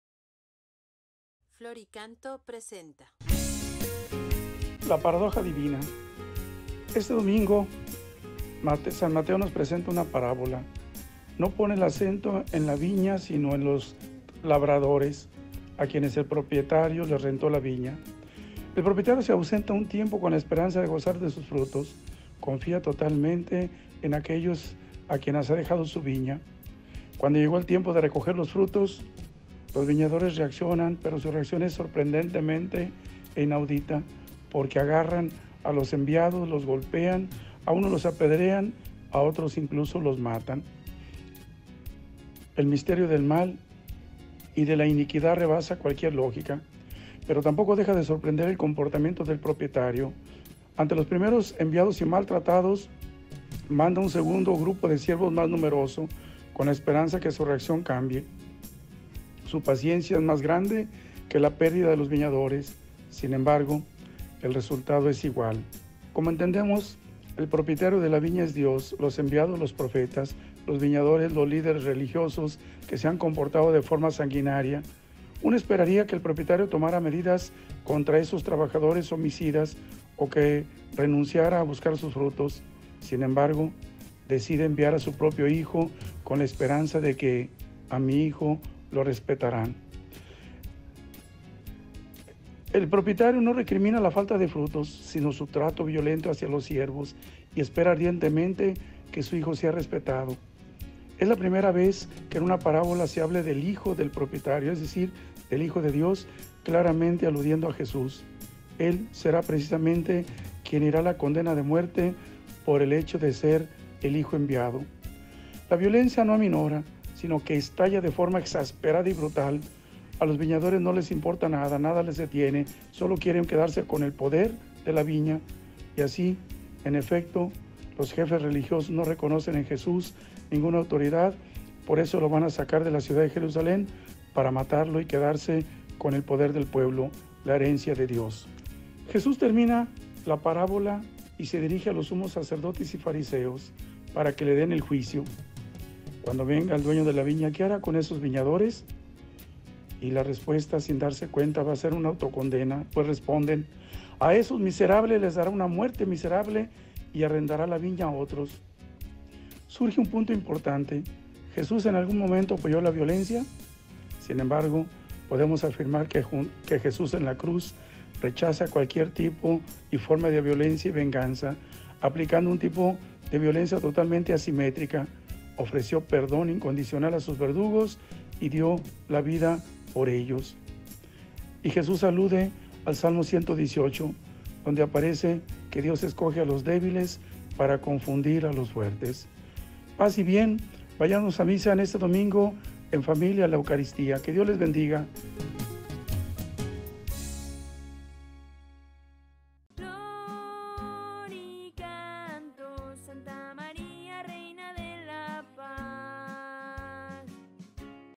Homilía dominical del 8 de octubre del 2023
Hoy nos comparte su mensaje dominical S. E. Mons. José Francisco González González, Obispo de la Diócesis de Campeche, quien nos invita a reflexionar y llevar a la vida cotidiana el Evangelio. ¿Que te dice a ti?